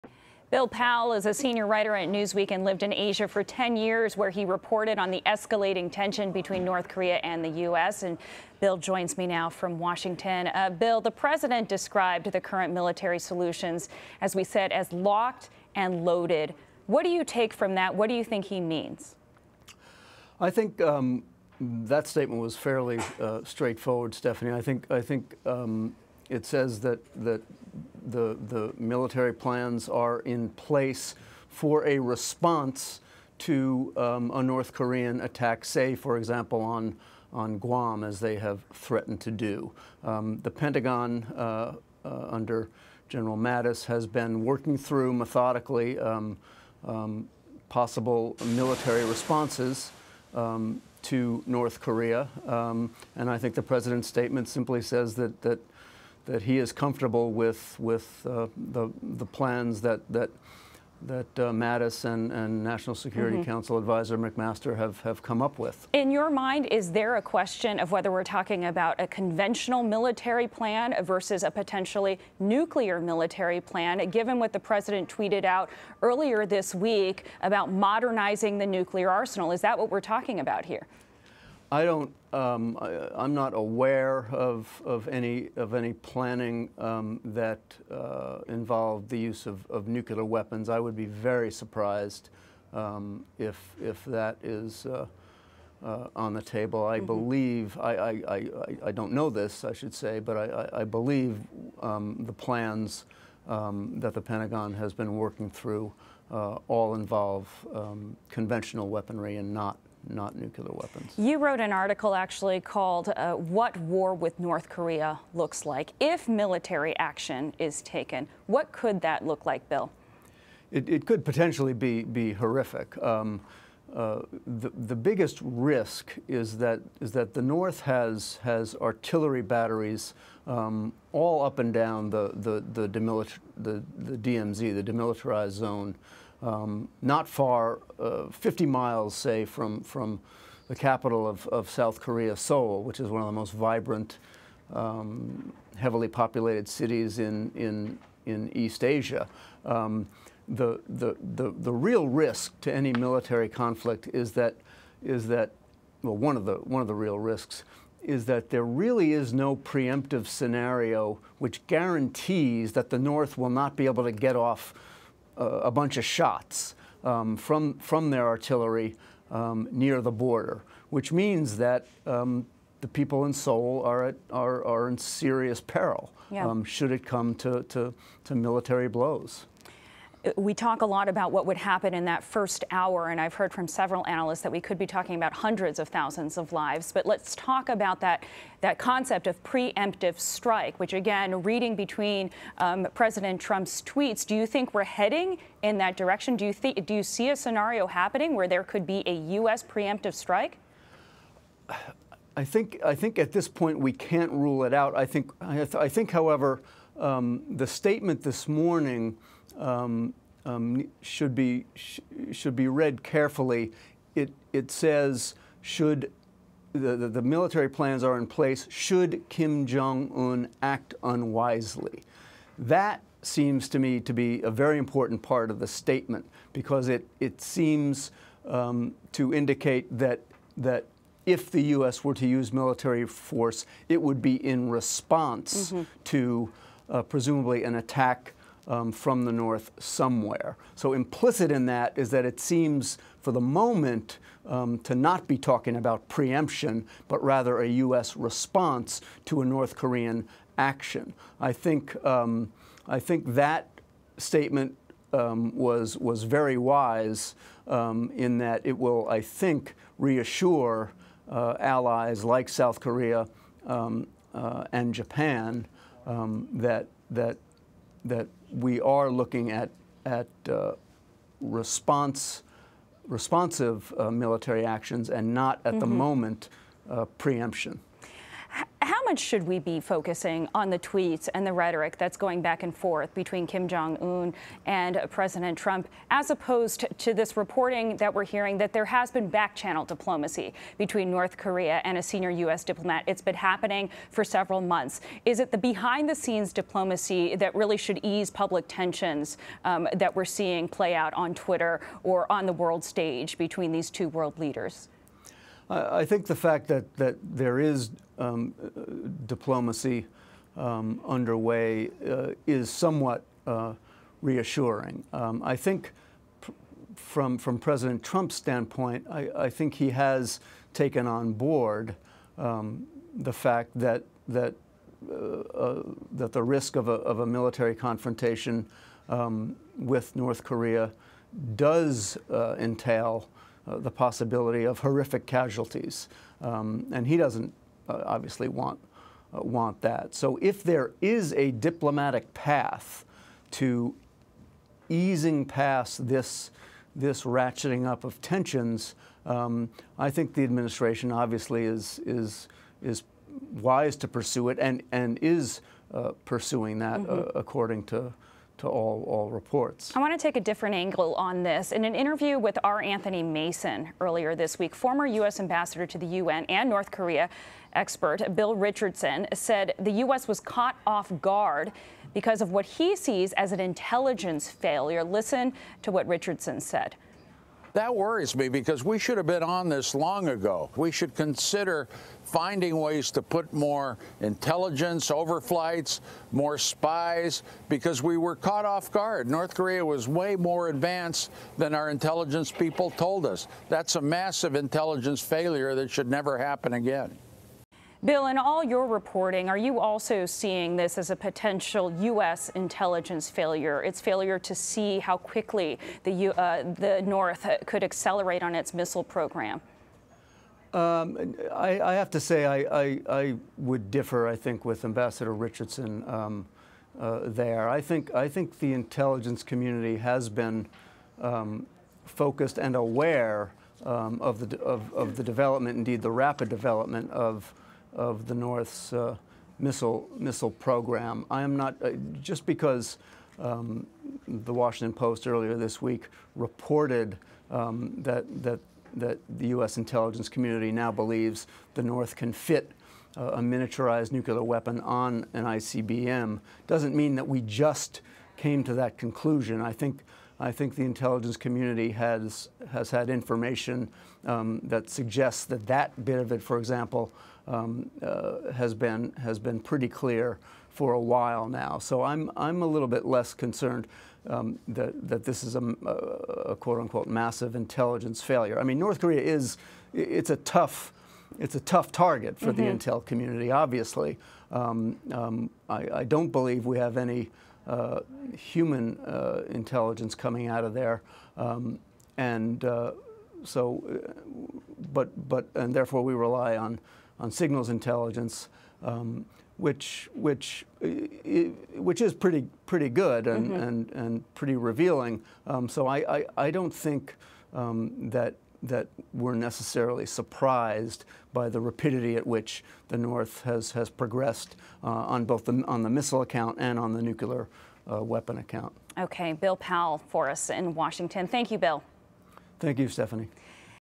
Trump says U.S. military is "locked and loaded"
President Trump ramped up his tough talk against North Korea, tweeting that the U.S. military is "locked and loaded" to respond to North Korea. Senior writer for Newsweek Bill Powell joins CBSN to discuss.